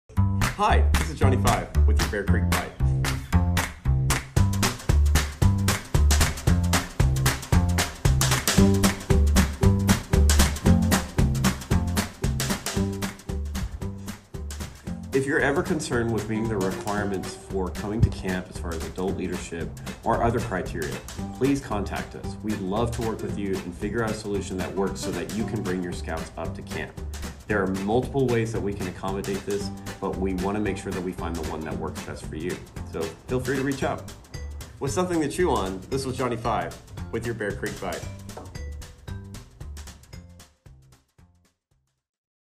Adult Leader Requirements for Bear Creek Summer Camp
As an adult leader coming to Bear Creek Scout Camp, there are a few things you'll want to make sure to check off your list early to make sure you're camp ready.